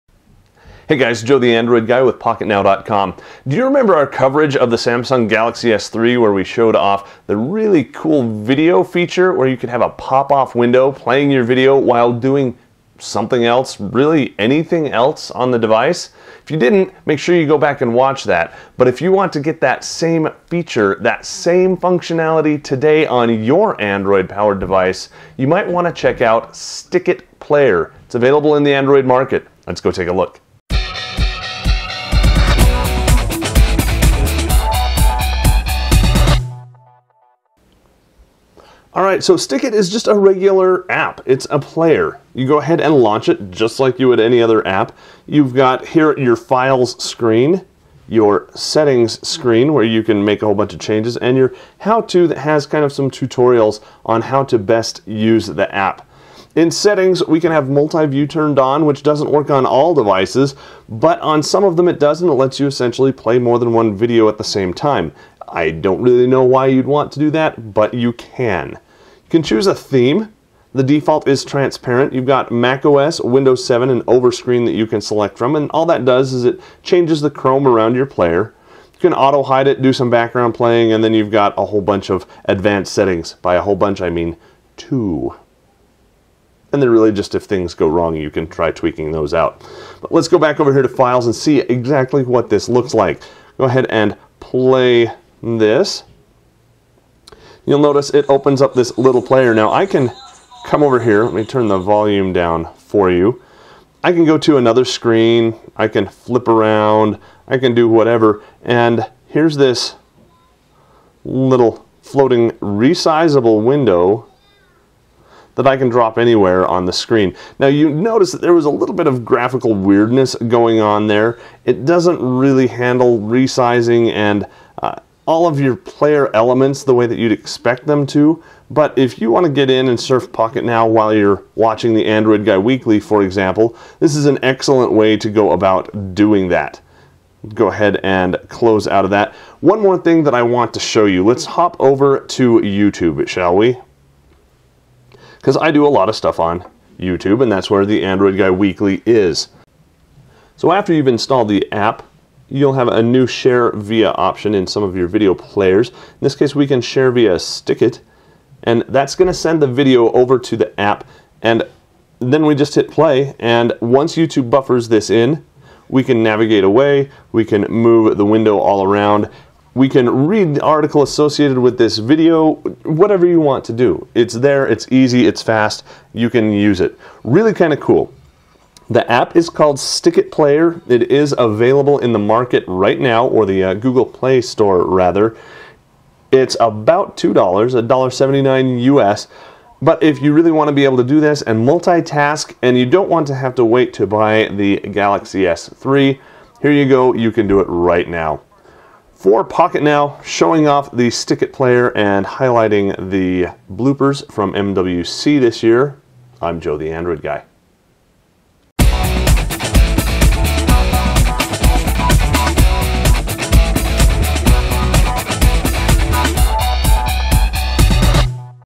Multi-tasking Picture-in-Picture Video Playback Using StickIt Player for Android | Pocketnow
If you remember our coverage of the Samsung Galaxy S III launch event you might recall a new video playback feature that lets you watch your videos in a pop-up window on top of whatever else you want to be doing (surfing the web, reading email, you get the idea).

StickIt for Android is a media player that you can download for your Android powered smartphone or tablet that lets you do exactly that, but you don't have to wait for the SGSIII. Want to see how it looks?